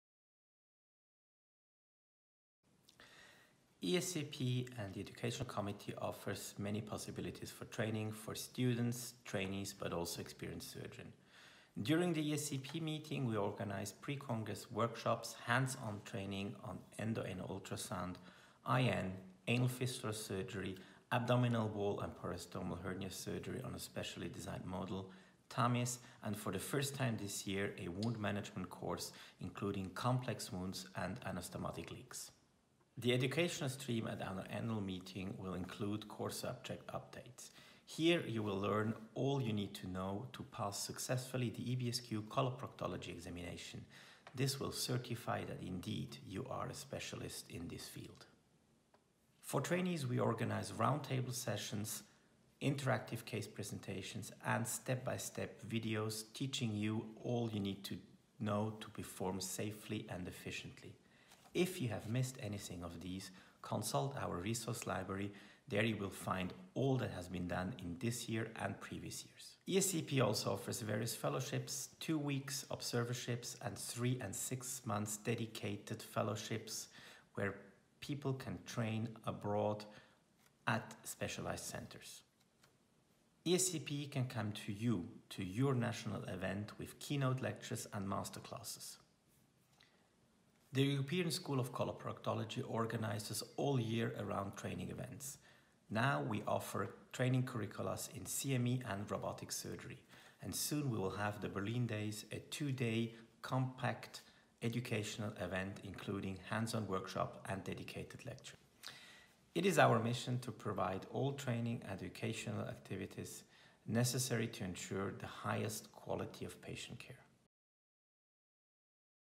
ESCPTrainingMonth - Dieter Hahnloser
Dieter discusses training opportunities available for ESCP members.